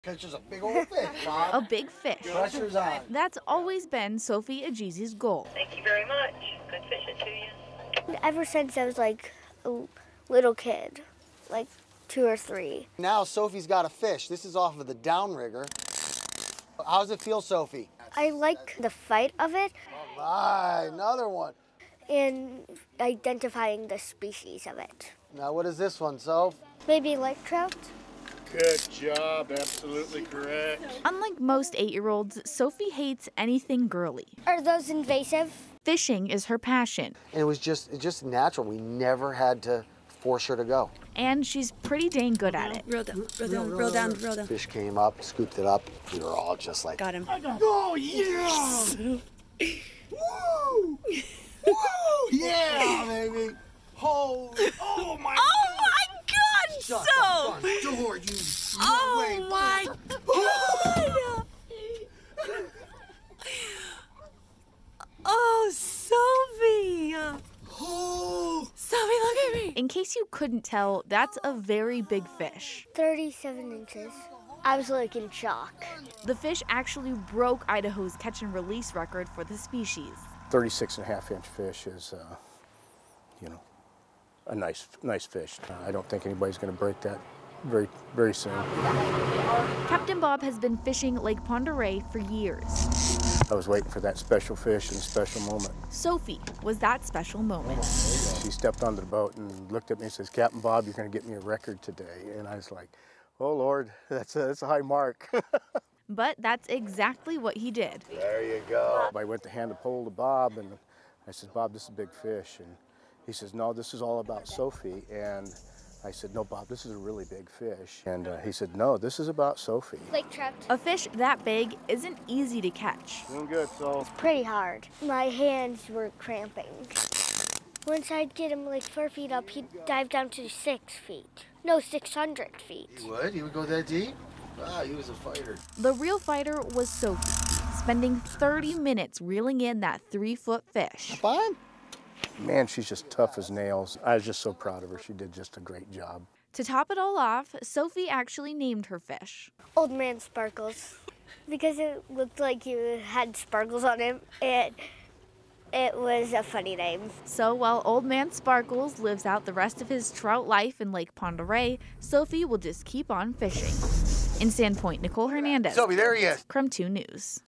'She's tough as nails': Sandpoint girl breaks Idaho record with 36.5-inch rainbow trout
Sophie Egizi caught the fish on Lake Pend Oreille in early October.